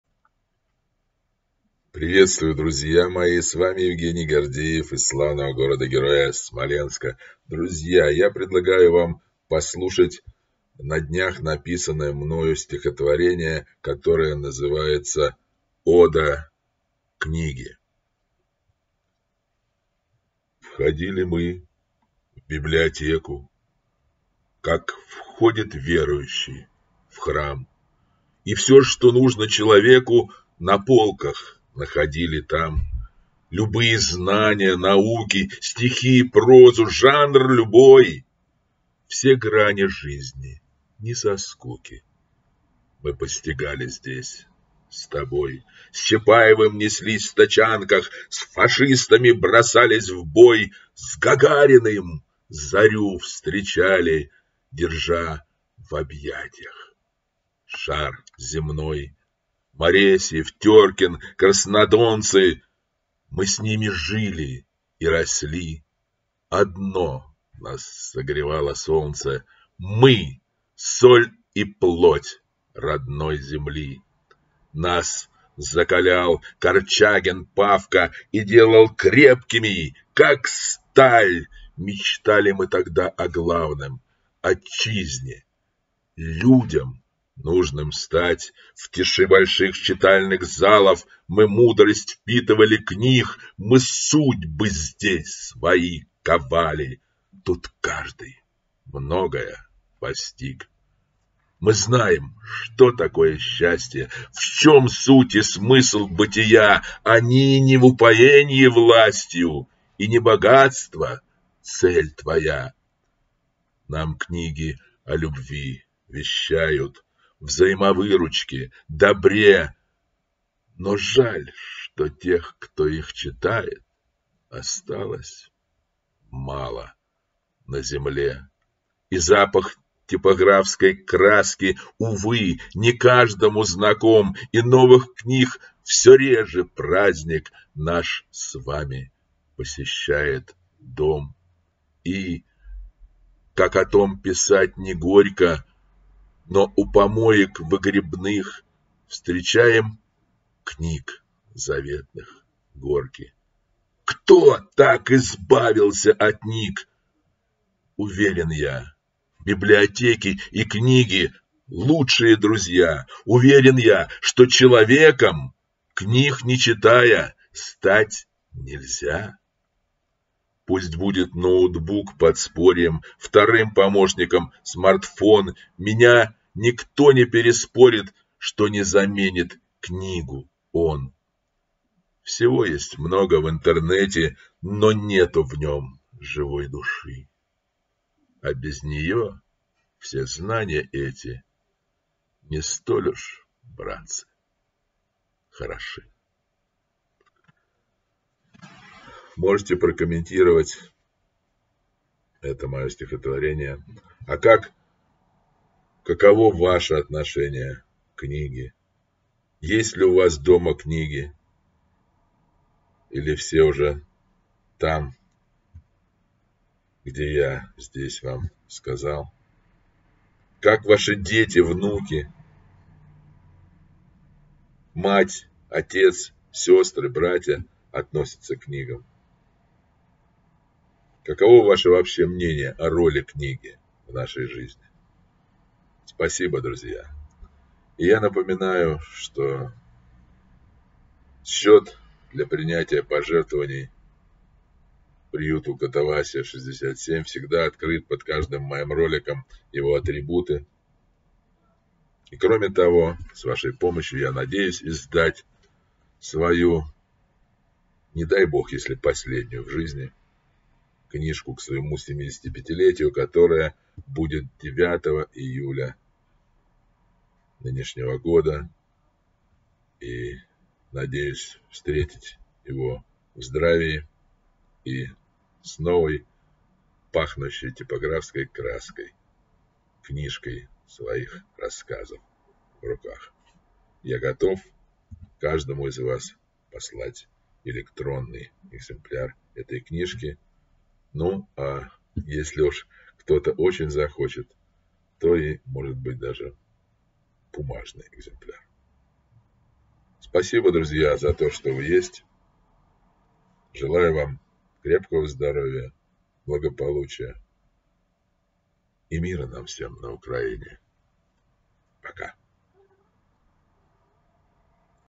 Ода книге. Приглашаю послушать моё новое стихотворение.
Для оказания помощи в издании моей книги и приюту "КотоВасия":
- карта Сбербанка  с привязкой к тел. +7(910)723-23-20